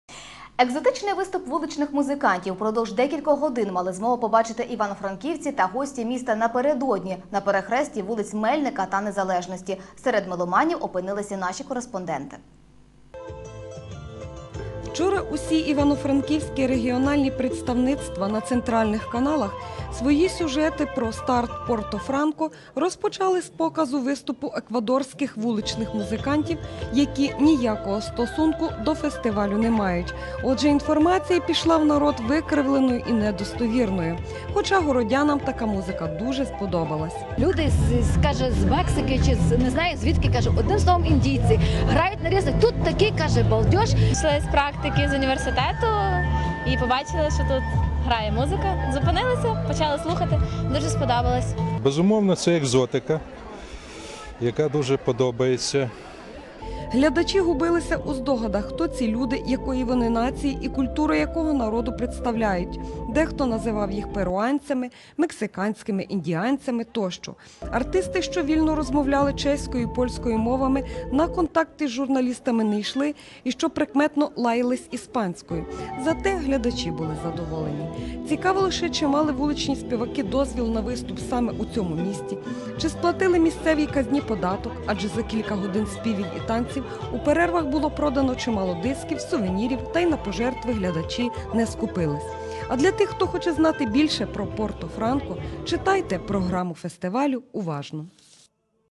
Еквадорці на «сотці»
Екзотичний виступ вуличних музикантів, впродовж кількох годин, мали змогу побачити Івано-Франківці та гості міста вчора на перехресті вулиць Мельника та Незалежності.